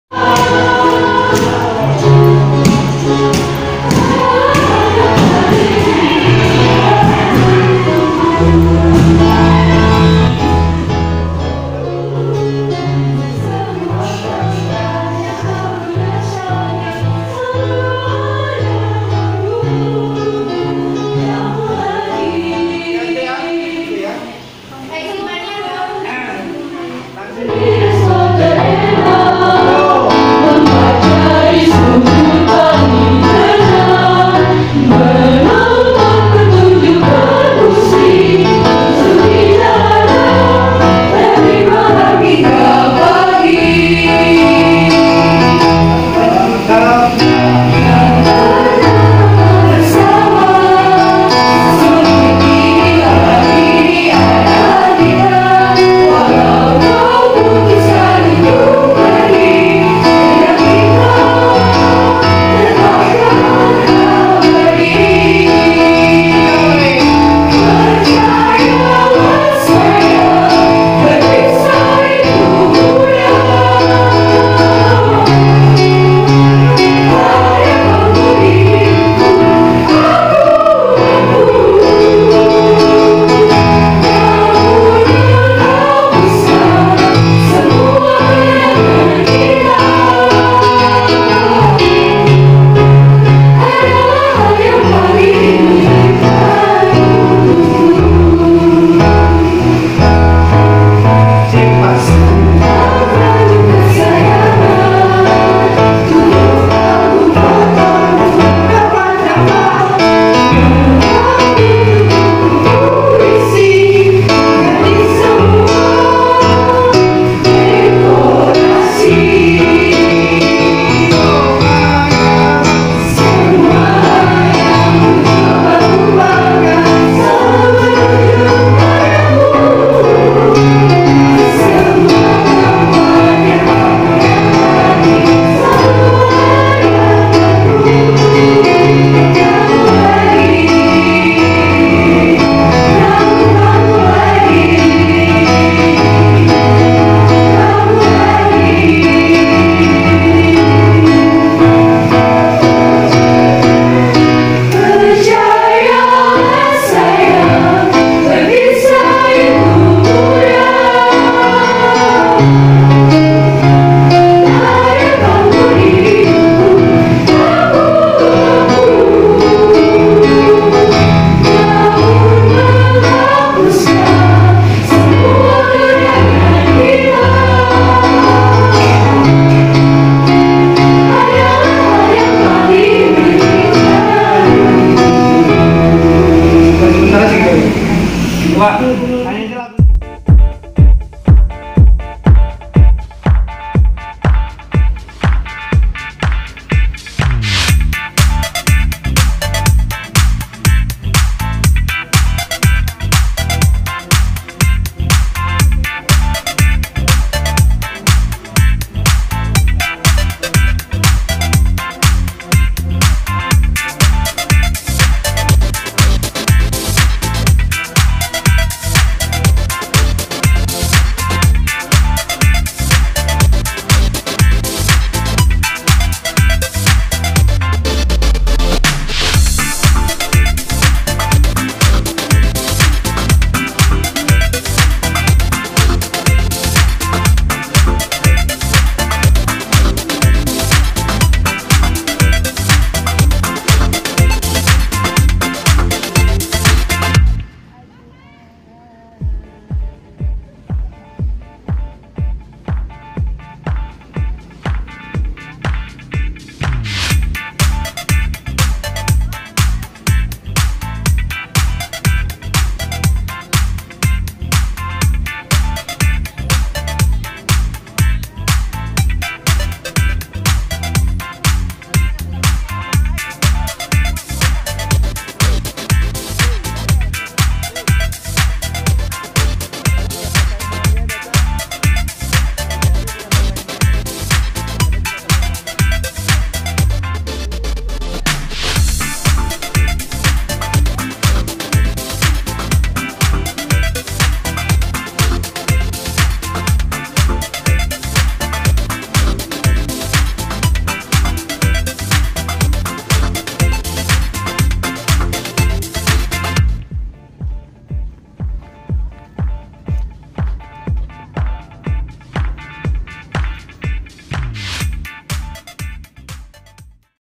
GEREBEG KEBISINGAN DI SORE HARI
Kegiatan latihan paduan suara atau lebih dikenal dengan BK VOICE ini kemarin sore

Tonton samapi habis ya gengs 🔥🔥🔥